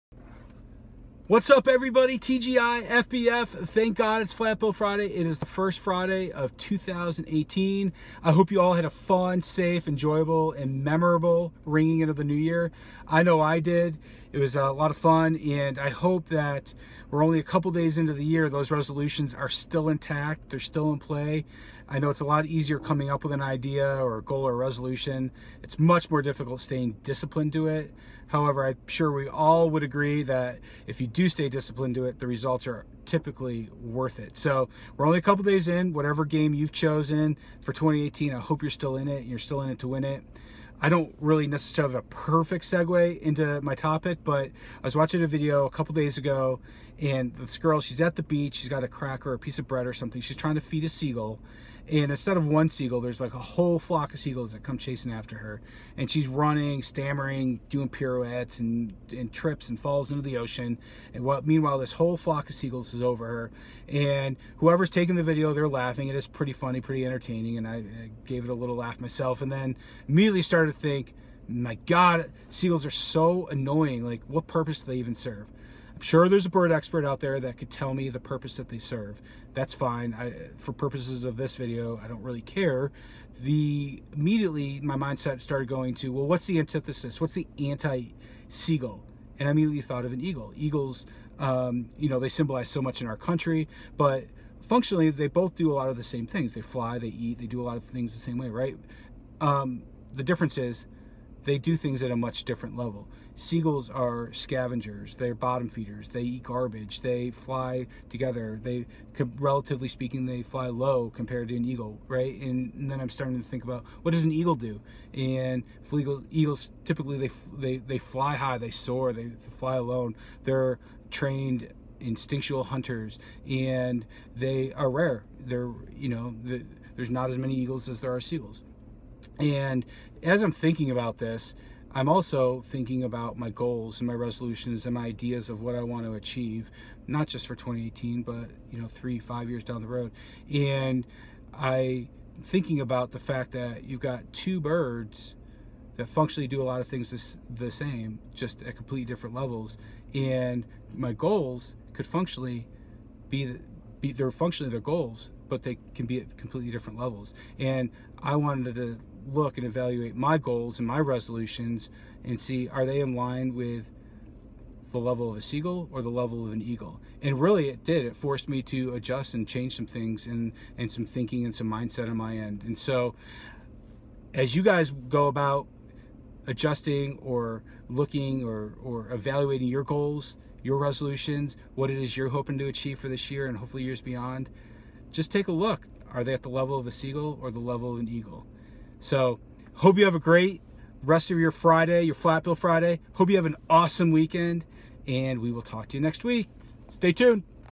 FlatBillFriday - 1/5/2018 - Seagull or Eagle?
FlatBillFriday - 1/5/2018 -  Eagle or Seagull? Both have similar functions but operate and achieve at completely different levels.

costello.re/FlatBillFriday